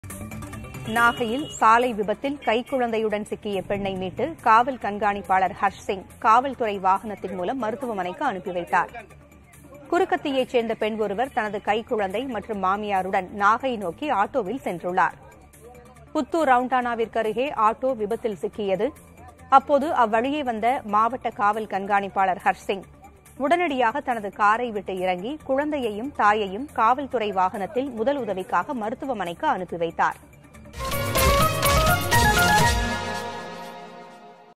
சாலை விபத்தில் கைக்குழந்தையுடன் சிக்கிய பெண்... ஓடோடி வந்து மீட்டகாவல் கண்காணிப்பாளர்! | PTT
காவல் கண்காணிப்பாளர்! | Nagapattinam | PTT